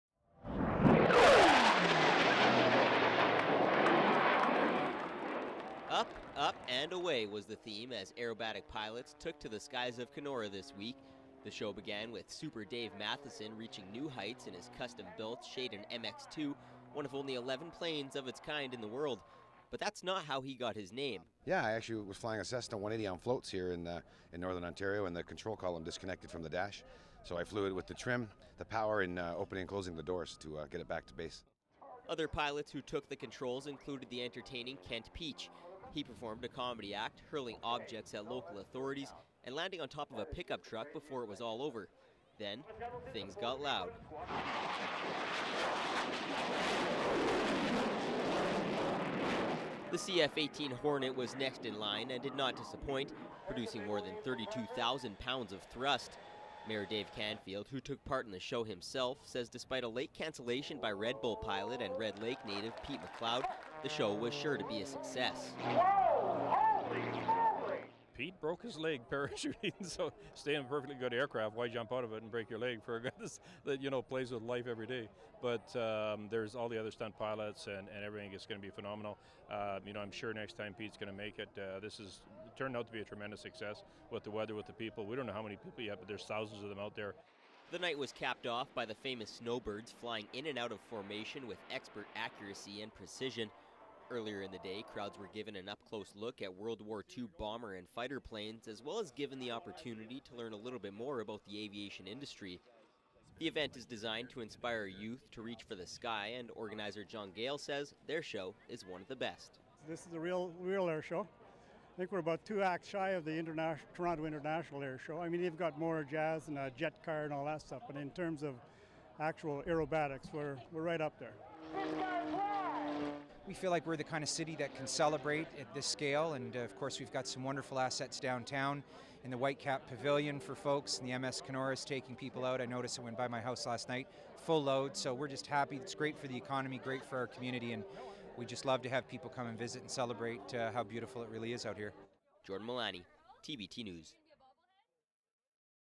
Kenora Airshow
Thousands of people flocked to the Kenora airport following the Canada Day weekend to take in the sights and the certainly loud sounds of the 2013 Air Show. The event featured an aerobatics show carried out by a number of acts, including a demonstration by a CF-18 Hornet and Canada's Air Force Aerobatics Team, the snowbirds.